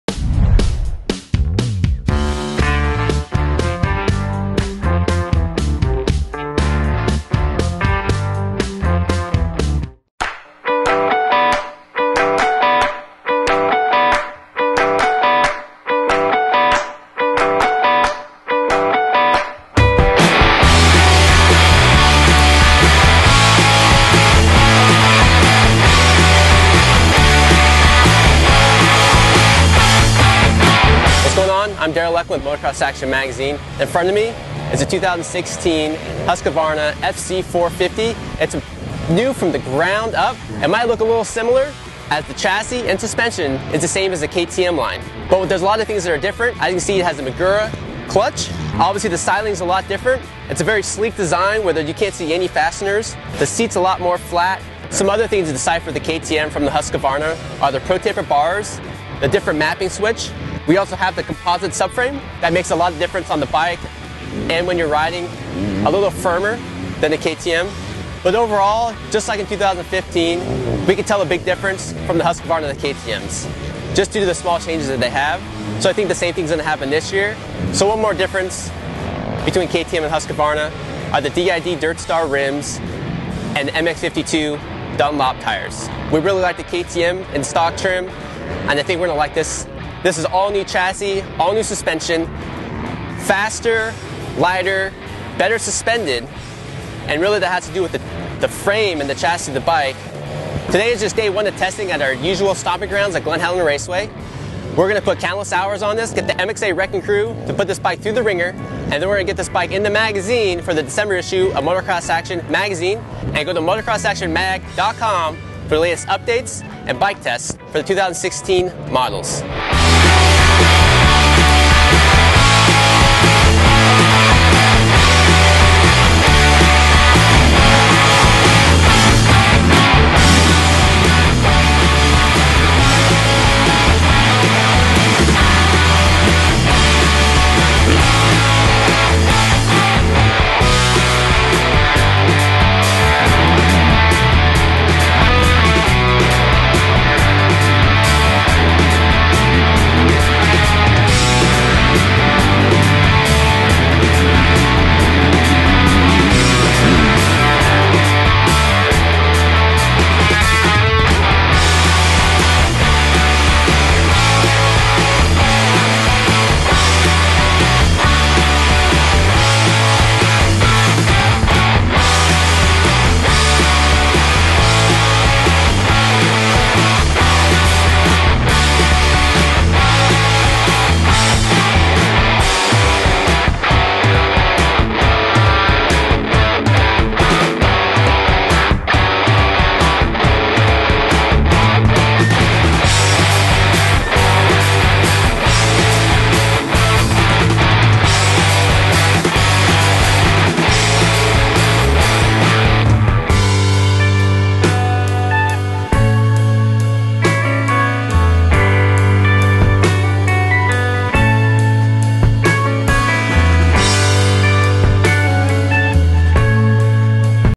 First Ride 2016 Husqvarna FC450 - Motocross Action Magazine
For this First Ride we throw a leg over the all new Husqvarna. This bike has been revamped from the ground up featuring an all new frame, new suspension, new air box/subframe design, map switches, electric start, new exhuast, and a whole lot more. Contrary to what people say the Husky and KTM are different in quite a few ways and we are here to show you how.